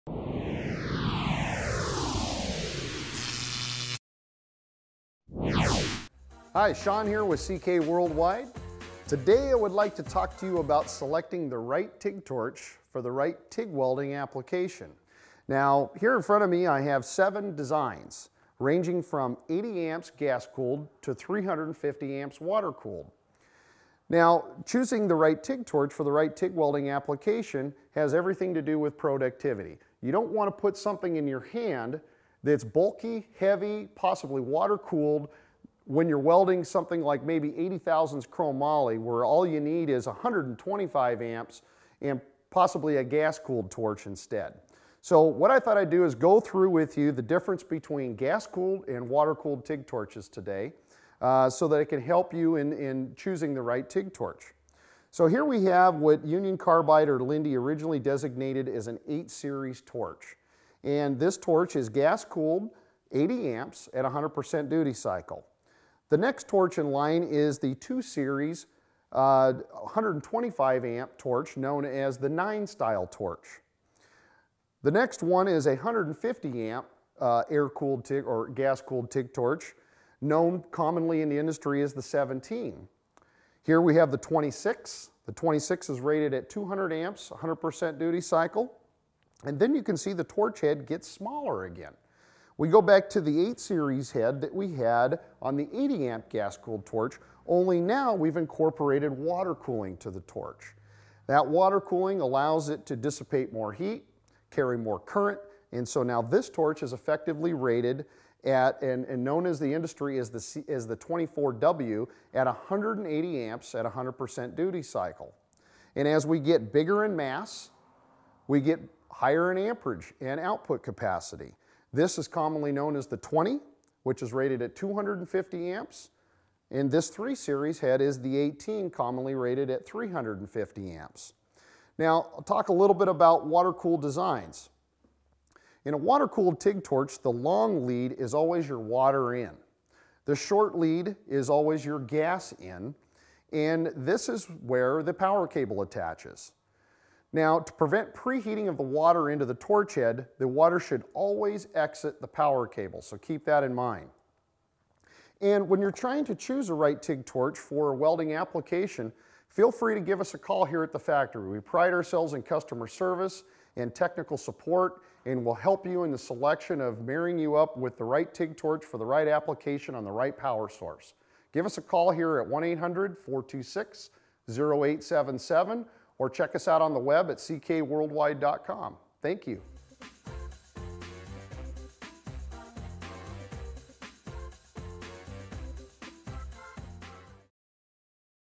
Gas Cooled vs Water Cooled TIG Torches
How do I know when to choose a Gas/Air Cooled TIG Torch vs. a Water Cooled TIG Torch?